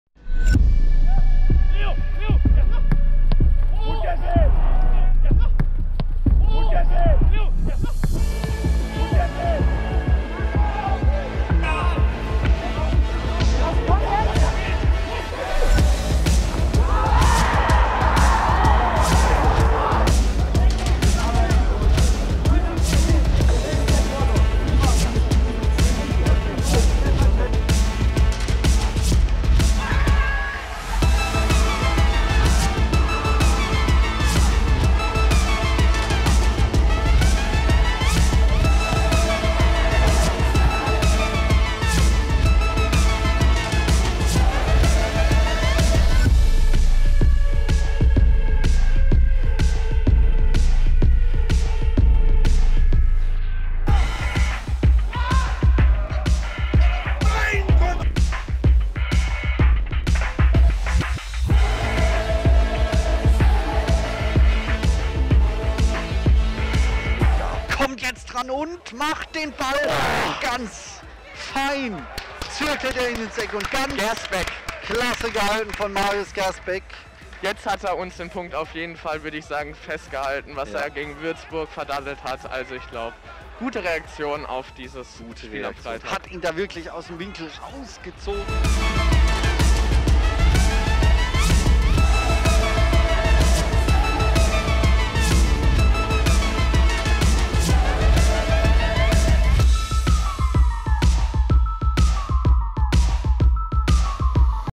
TOP 7 - Aktionen von Marius Gersbeck 20/21
Wer ist auf diese Reihenfolge gekommen?! Diskutiert gerne in den Kommentaren über Eure Lieblingsszene der vergangenen Spielzeit. In der nächsten Saison hoffentlich wieder mit Euch im Stadion. Jetzt Dauerkarte im BBBank Wildpark sichern.